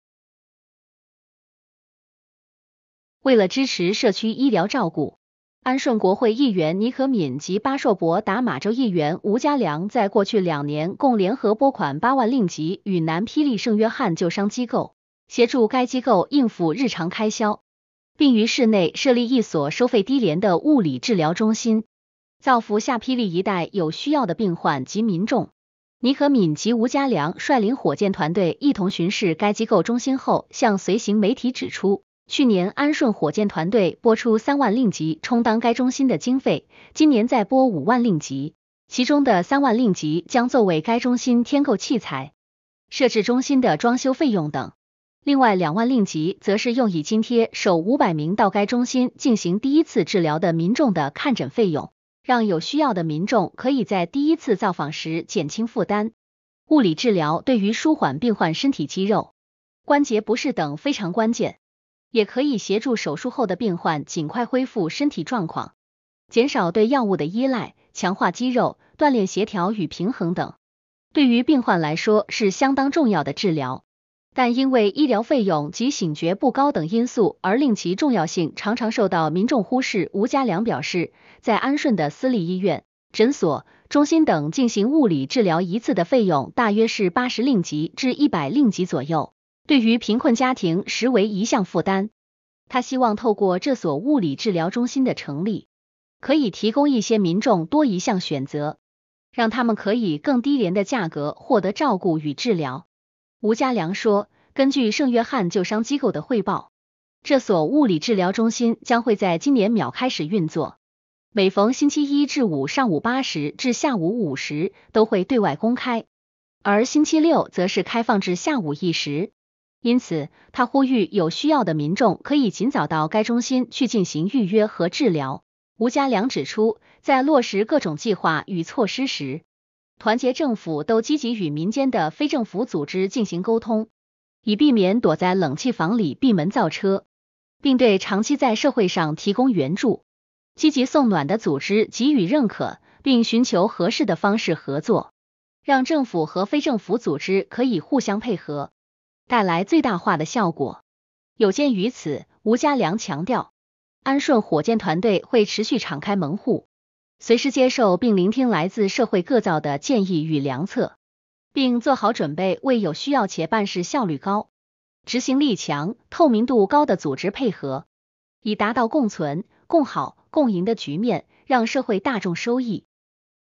两年拨8万助南霹圣约翰 安顺火箭冀强化社区医疗体系
两年拨8万助南霹圣约翰
安顺火箭冀强化社区医疗体系

为了支持社区医疗照顾，安顺国会议员倪可敏及巴硕伯打玛州议员吴家良在过去两年共联合拨款8万令吉予南霹雳圣约翰救伤机构，协助该机构应付日常开销，并于市内设立一所收费低廉的物理治疗中心，造福下霹雳一带有需要的病患及民众。

安顺火箭减轻病患者负担

倪可敏及吴家良率领火箭团队一同巡视该机构中心后向随行媒体指出，去年安顺火箭团队拨出3万令吉充当该中心的经费，今年再拨5万令吉，其中的3万令吉将作为该中心添购器材、设置中心的装修费用等，另外2万令吉则是用以津贴首500名到该中心进行第一次治疗的民众的看诊费用，让有需要的民众可以在第一次造访时减轻负担。

“物理治疗对于舒缓病患身体肌肉、关节不适等非常关键，也可以协助手术后的病患尽快恢复身体状况、减少对药物的依赖、强化肌肉、锻炼协调与平衡等，对于病患来说是相当重要的治疗，但因为医疗费用及醒觉不高等因素而令其重要性常常受到民众忽视。”

提供民众低廉选择

吴家良表示，在安顺的私立医院、诊所、中心等进行物理治疗一次的费用大约是80令吉至100令吉左右，对于贫困家庭实为一项负担，他希望透过这所物理治疗中心的成立，可以提供一些民众多一项选择，让他们可以更低廉的价格获得照顾与治疗。

吴家良说，根据圣约翰救伤机构的汇报，这所物理治疗中心将会在今年杪开始运作，每逢星期一至五上午8时至下午5时都会对外公开，而星期六则是开放至下午1时，因此他呼吁有需要的民众可以尽早到该中心去进行预约和治疗。

积极与非政府组织合作

吴家良指出，在落实各种计划与措施时，团结政府都积极与民间的非政府组织进行沟通，以避免躲在冷气房里闭门造车，并对长期在社会上提供援助、积极送暖的组织给予认可，并寻求合适的方式合作，让政府和非政府组织可以互相配合，带来最大化的效果。

有鉴于此，吴家良强调，安顺火箭团队会持续敞开门户，随时接受并聆听来自社会各造的建议与良策，并做好准备为有需要且办事效率高、执行力强、透明度高的组织配合，以达到共存、共好、共赢的局面，让社会大众收益。